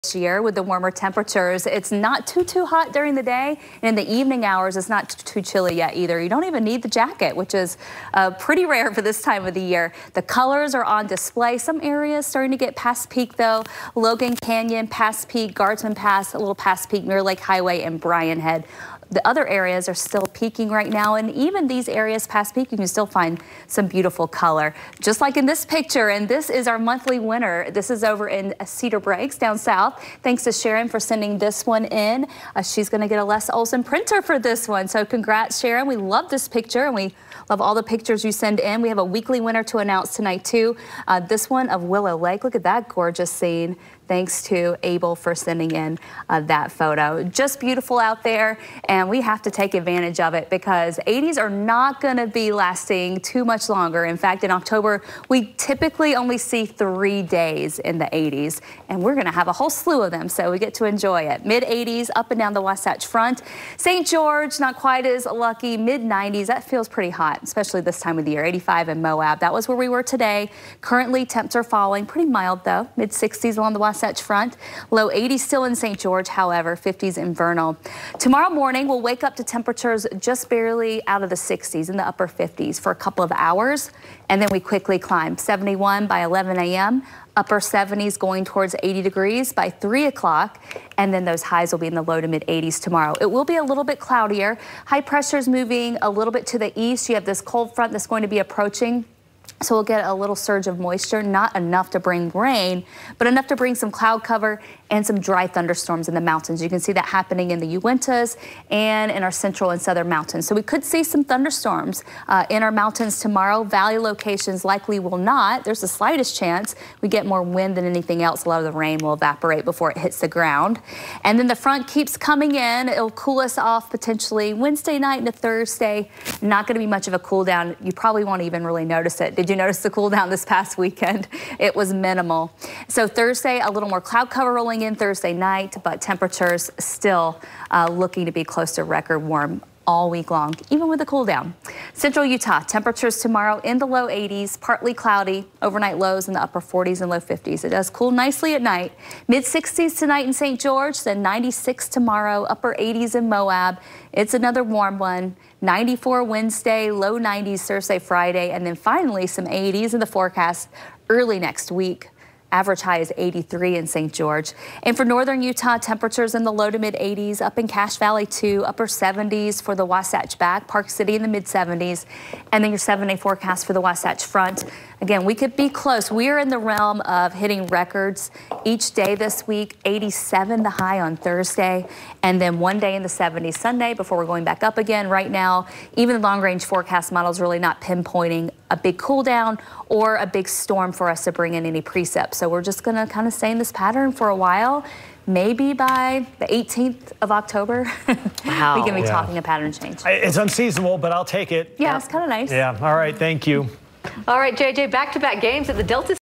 Evening weather forecast for Oct. 7, 2024
Expect highs in the mid 80s each day this work week with lows in the upper 50s. There's a slight chance for a few mountain storms Tuesday afternoon, but otherwise we have zero rain in the forecast for the rest of this week.